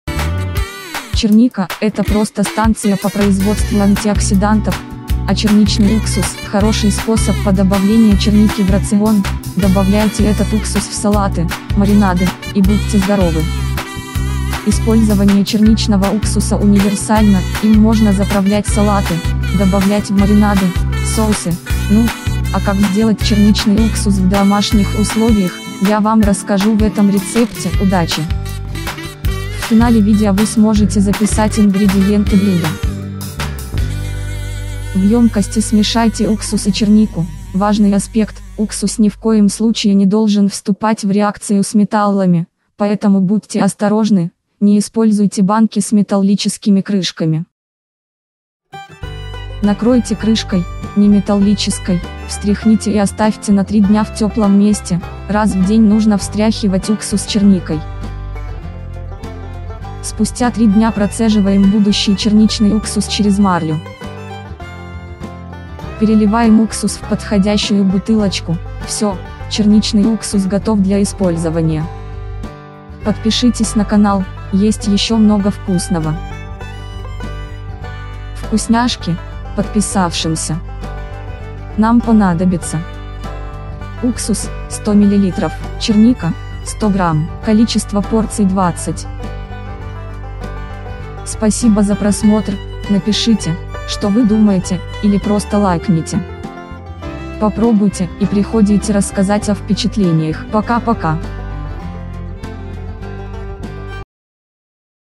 ★ЭТО ВКУСНО!★ ЧЕРНИЧНЫЙ УКСУС ★
★Вы влюбитесь во вкус! Черника - это просто станция по производству антиоксидантов, а черничный уксус - хороший способ по добавлению черники в рацион● Добавляйте этот уксус в салаты, маринады - и будьте здоровы●